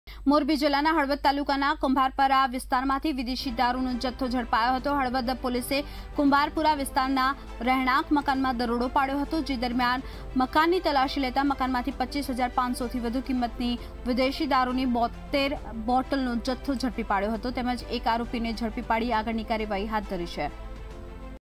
25 હજાર 500થી વધુ કિંમતનો મુદ્દામાલ જપ્ત | Goods worth more than Rs 25,500 seized
હળવદ તાલુકાના કુંભારપરા વિસ્તારમાંથી વિદેશી દારૂ ઝડપાયો
હળવદ પોલીસનો કુંભારપરા વિસ્તારના રહેણાંક મકાનમાં દરોડો
25 હજાર 500થી વધુ કિંમતનો મુદ્દામાલ જપ્ત
વિદેશી દારૂની 72 બોટલો ઝડપાઈ
એક આરોપીની કરી અટકાયત